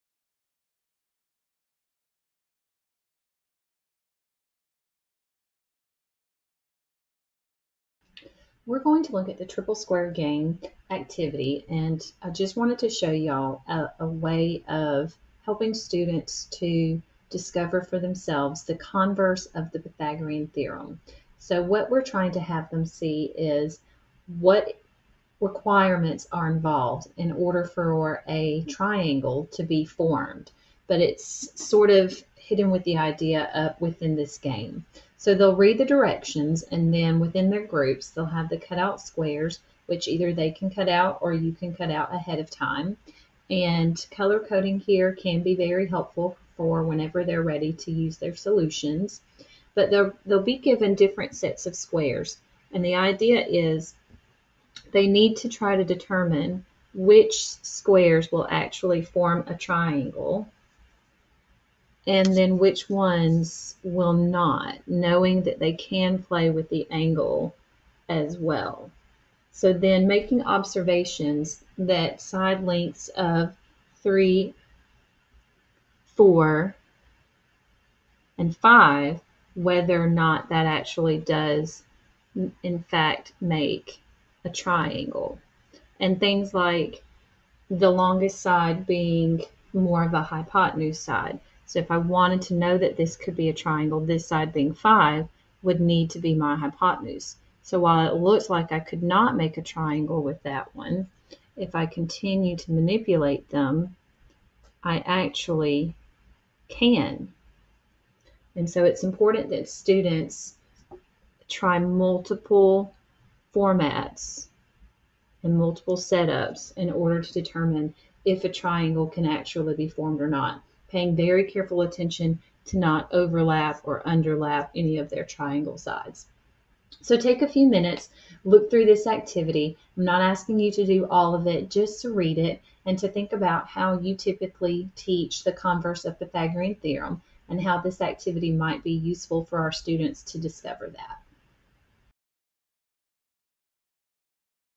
Triple Square Game
This video is an introduction to the Triple Square Game in which the Converse of the Pythagorean Theorem is derived.